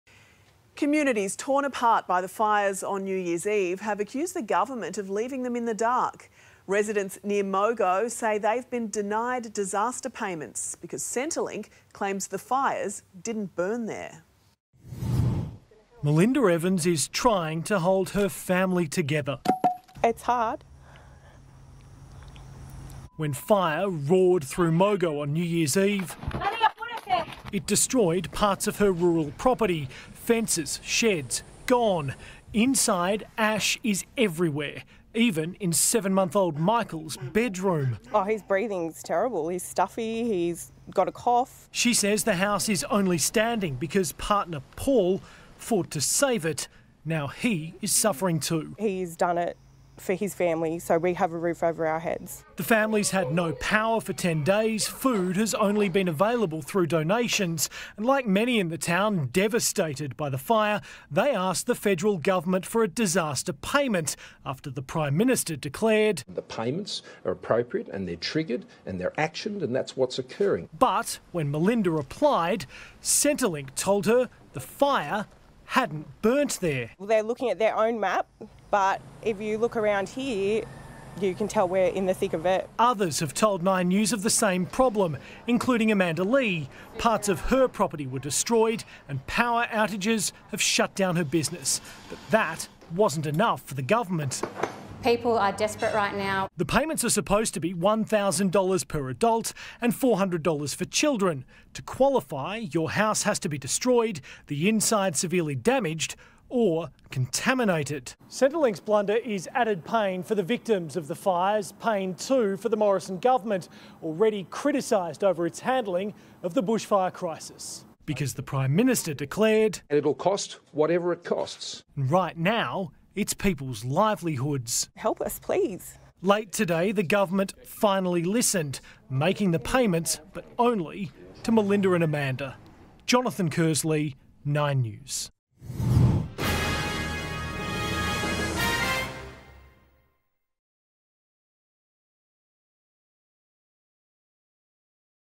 NSW Bushfires: Centrelink denies paying bushfire victims | Nine News Australia
Residents near Mogo on the NSW south coast claim they have been denied disaster payments because Centrelink claims the fire didn’t burn there.